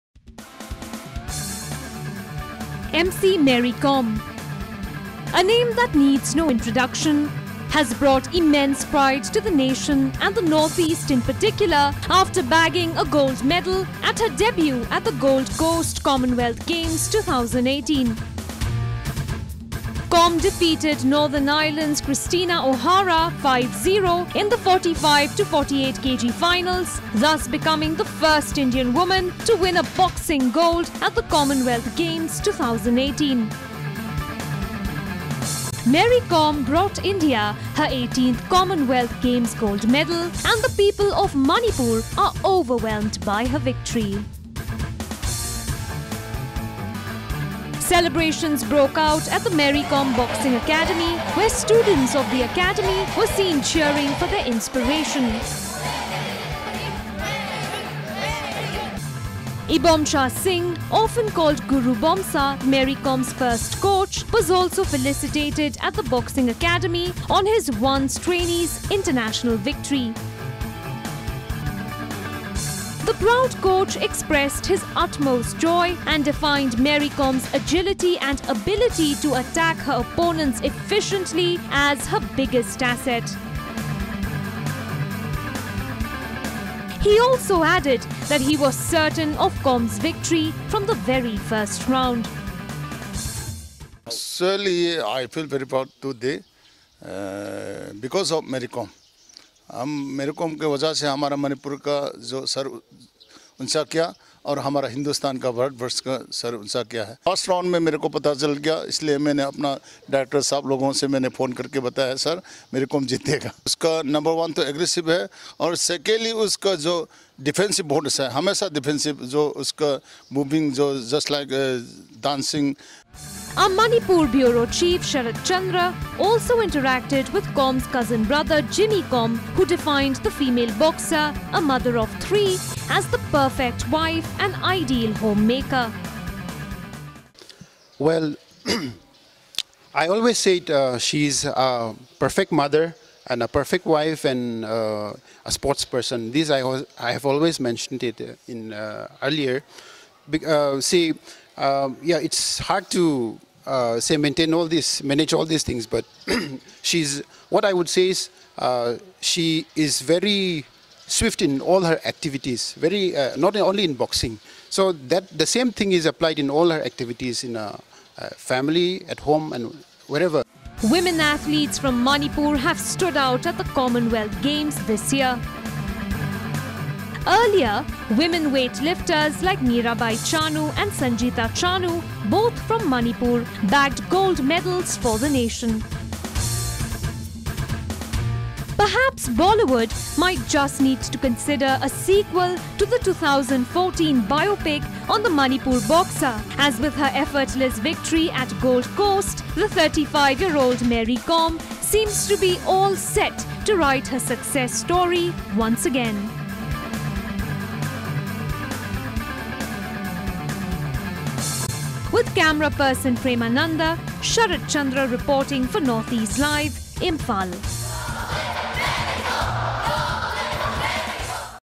Magnificent Mary Kom
Manipur's MC Mary Kom bagged her maiden Commonwealth Games  gold in the women's 45-48 kg category at the ongoing Commonwealth Games.